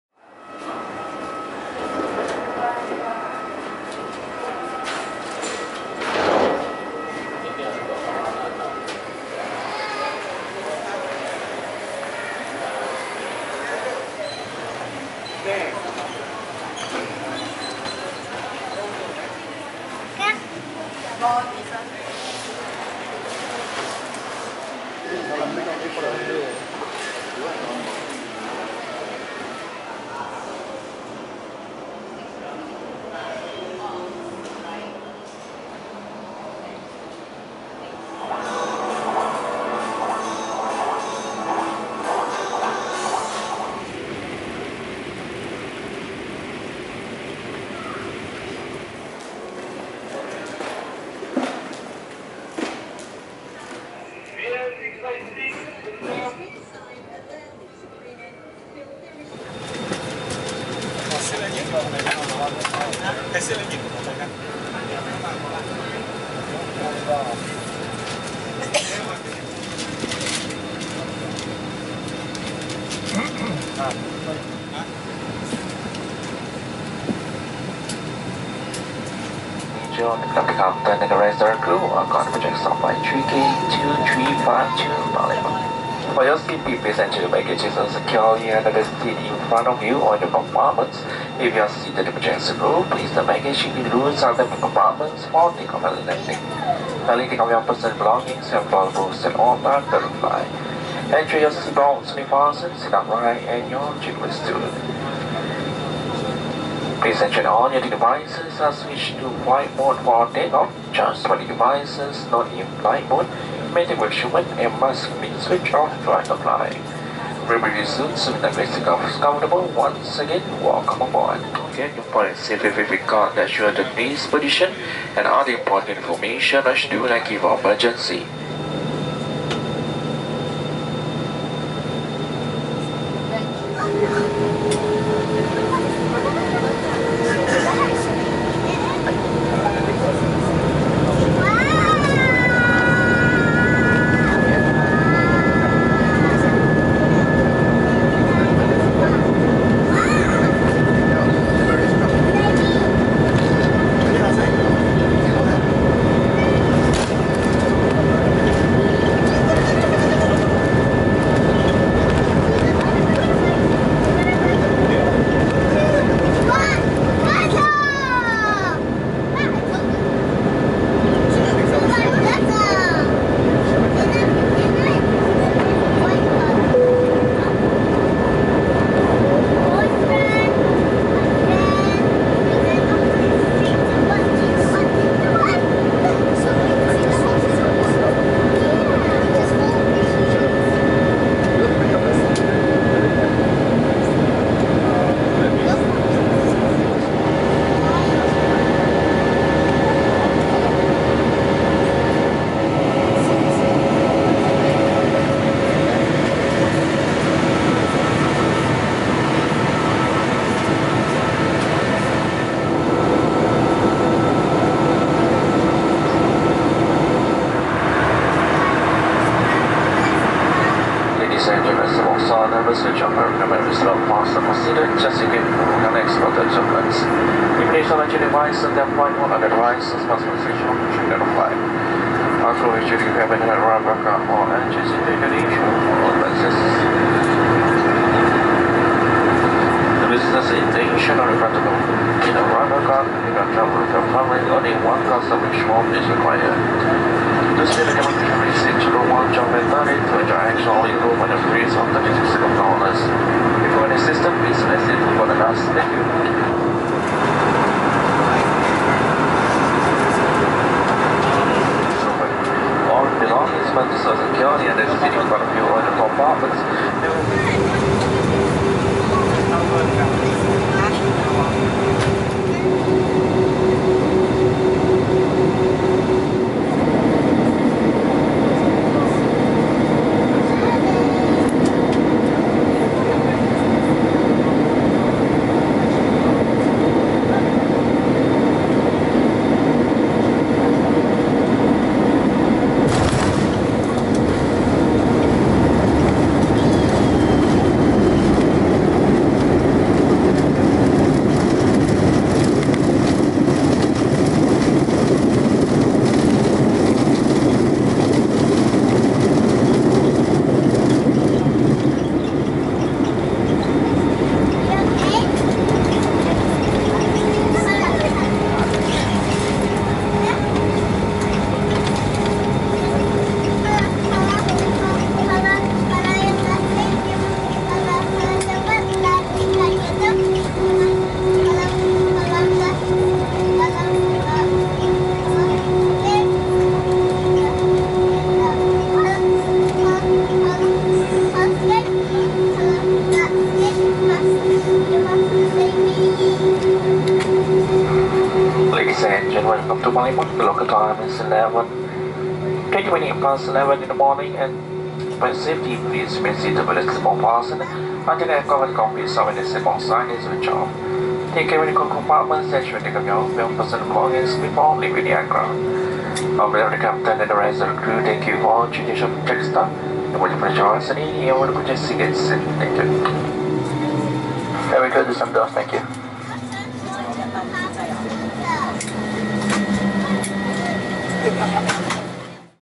🇸🇬🇮🇩 Jetstar Asia A320 Flight Experience: 3K235 Singapore → Palembang
A short flight onboard Jetstar Asia from Singapore to Palembang, Indonesia (South Sumatra).

It was a short flight from Singapore to Palembang with less than 1 hour flying time. No time for the inflight meal purchase service as the fasten seatbelt sign remain switched on (encountering a bit of turbulence) until halfway when the skies are getting brighter.